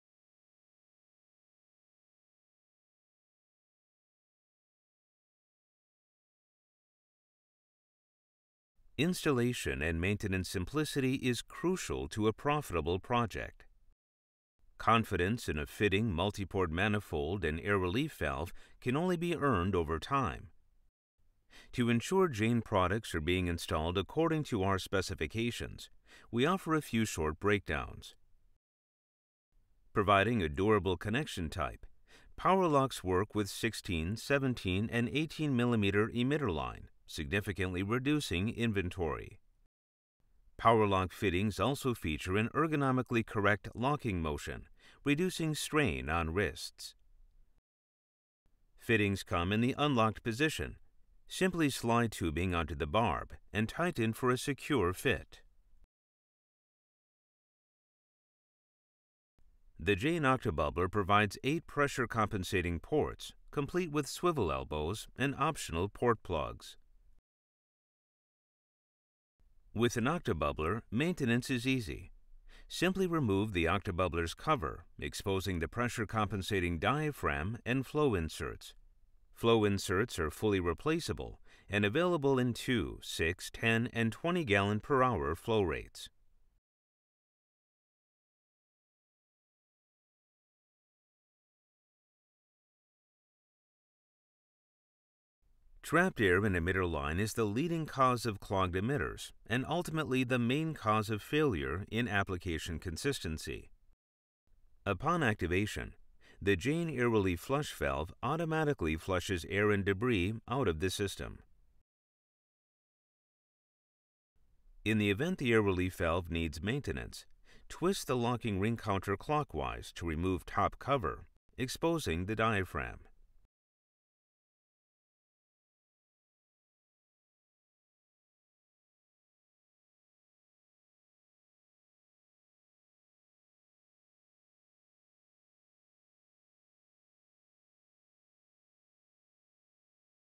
JAIN Irrigation Product Training Pt 1
JAIN Irrigation knows proper training and product explanation is imperative to the landscape irrigation industry. We want installers to feel as comfortable installing a product as we do selling our products. In this first part of a multi-part series we are discussing proper maintenance procedures for our air relief valves, filters and popular Octa-Bubbler manifold.